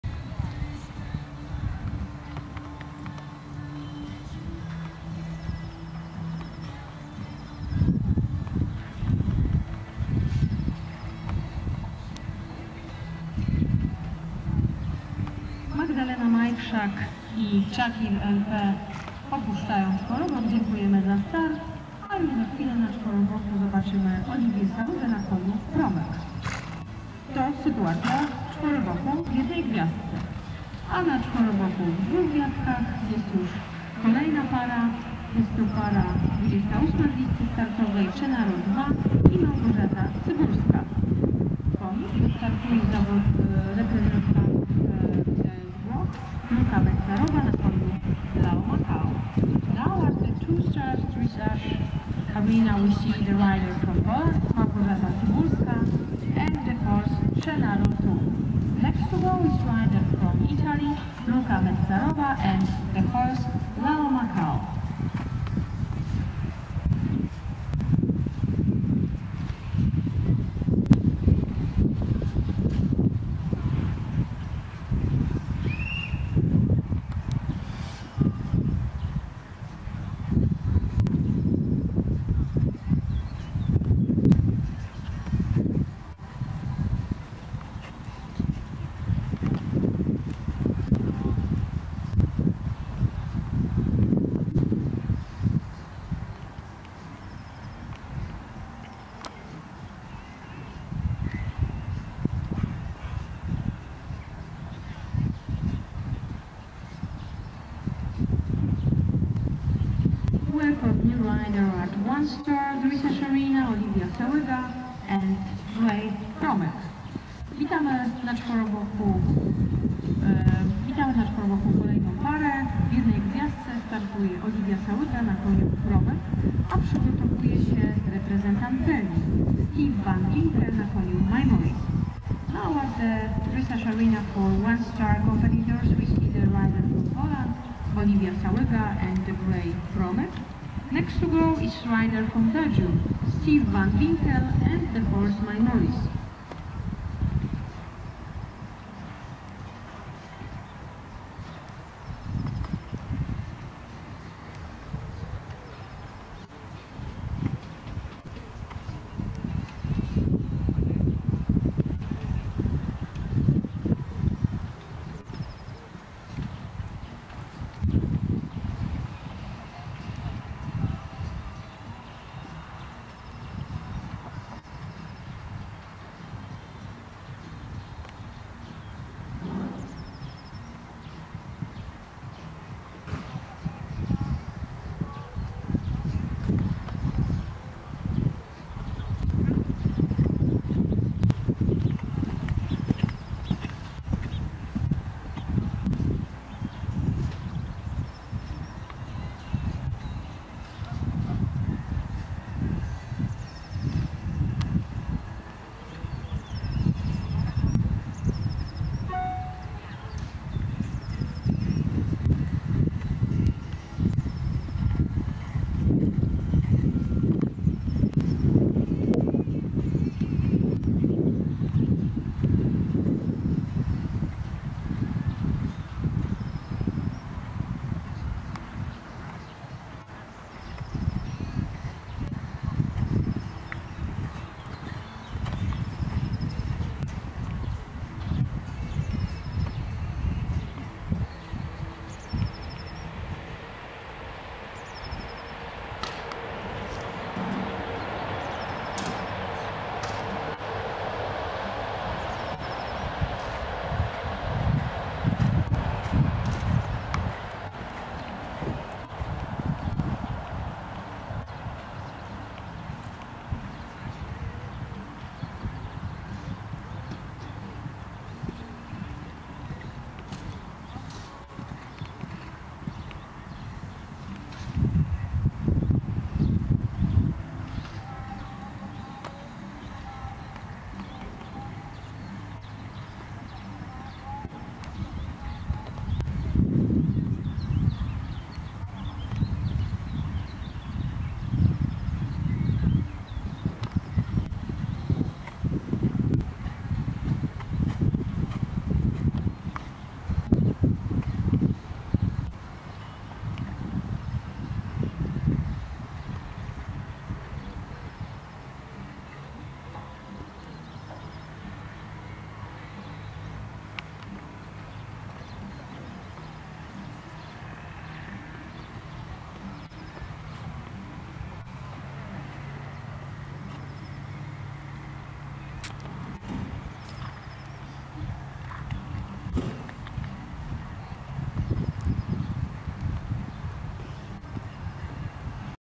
Gosia Cybulska na Chenaro 2, pierwsze CIC 2* Sopot
Wynik tego programu to 40,2, drugie miejsce na 41 zawodników